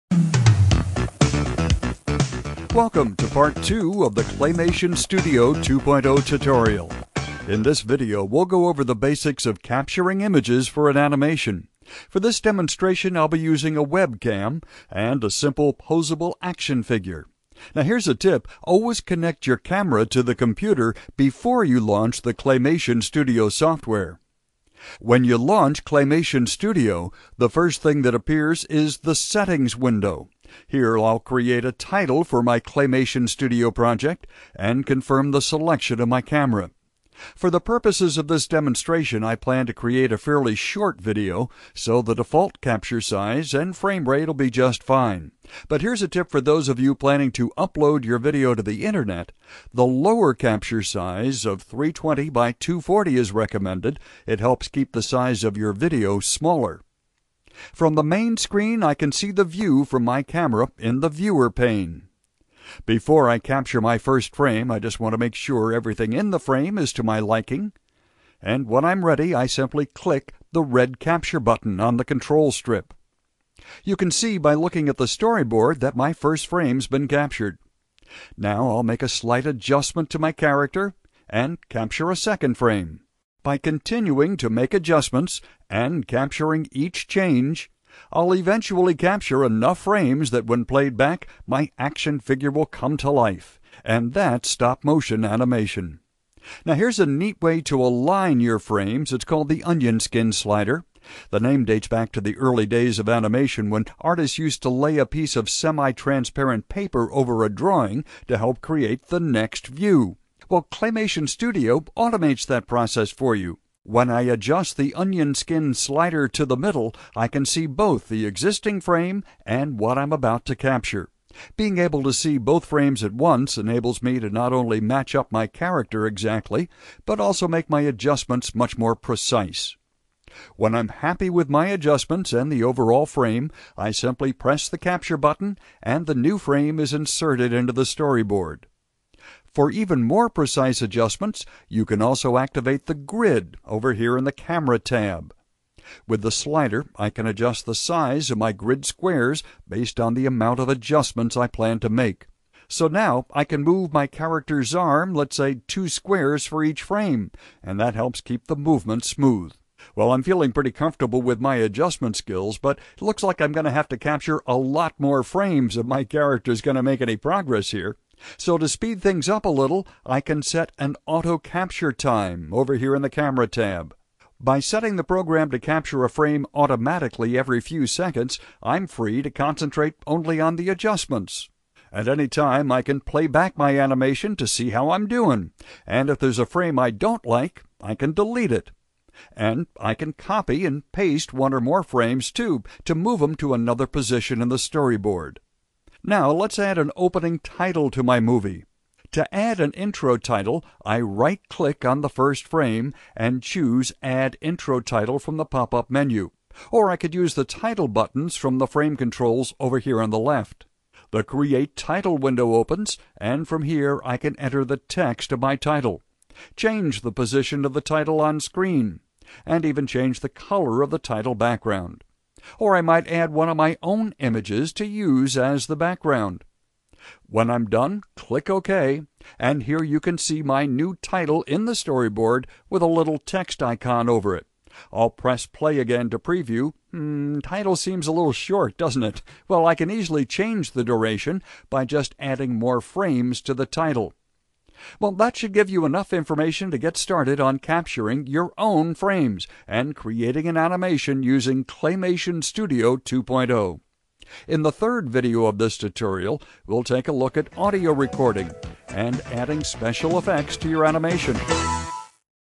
honestech Claymation Studio 2.0 Tutorial - Part 2/3
Video tutorial on honestech Claymation Studio 2.0 (Part 2 of 3)